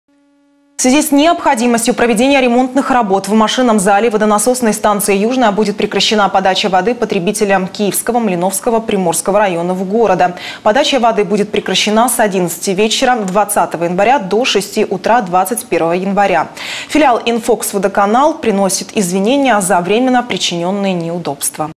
Ночью у жителей трех районов Одессы не будет воды
В связи с необходимостью проведения ремонтных работ в машинном зале водонасосной станции «Южная» будет прекращена подача воды потребителям Киевского, Малиновского, Приморского районов города. Подача воды будет прекращена с 23:00 20 января до 6:00 21-го. Филиал «Инфоксводоканала» приносит извинения за временно причиненные неудобства.